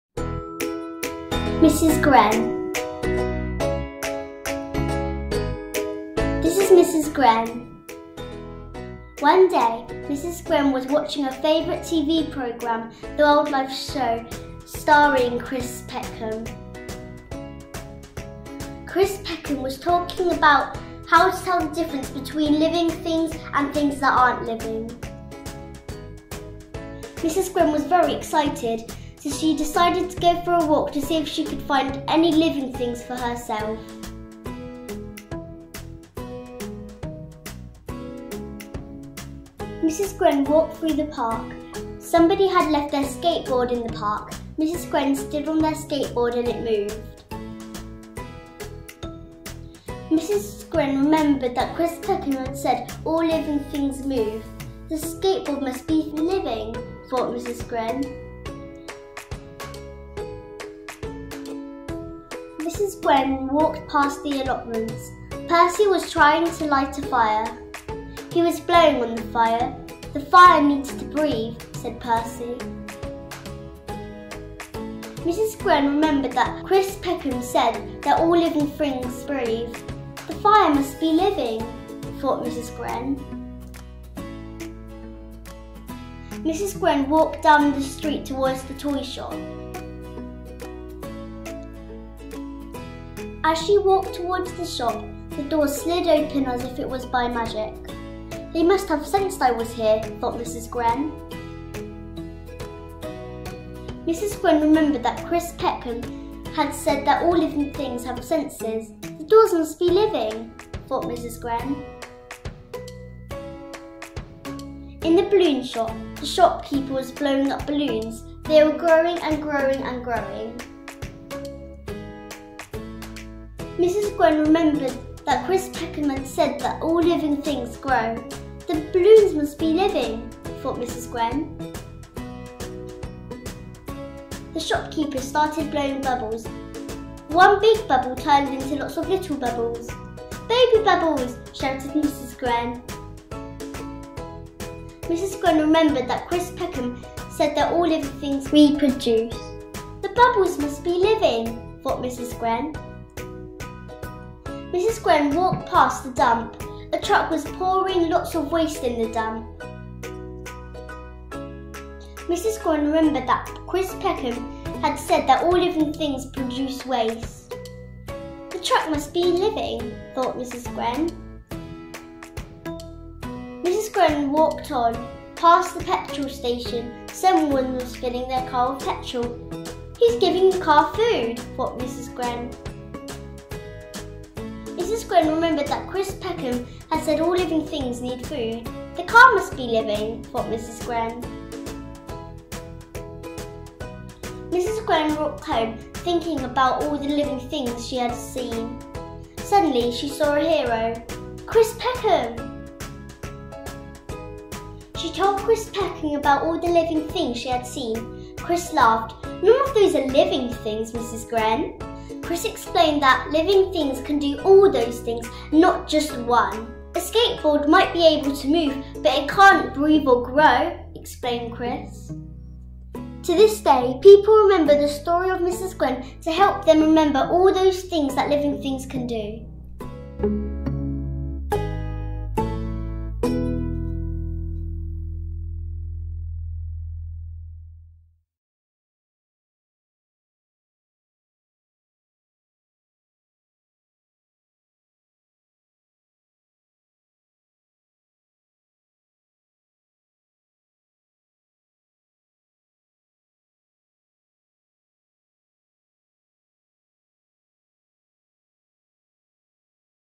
Mrs Gren (Year Two Science - Brilliant Habitats)
Learn about the characteristics of living things with Mrs Gren